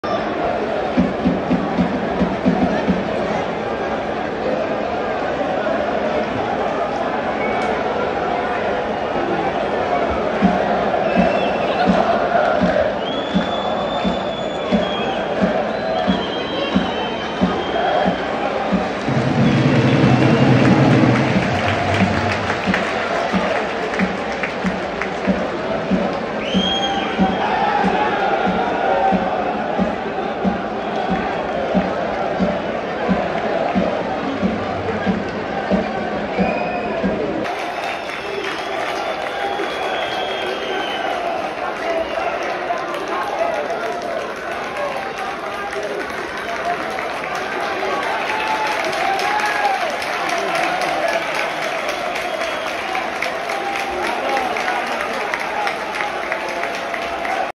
Aripa tanara de huligani a Peluzei Nord Rapid invita aradenii la fairfight acestia refuzand de frica
la Rapid - UTA Arad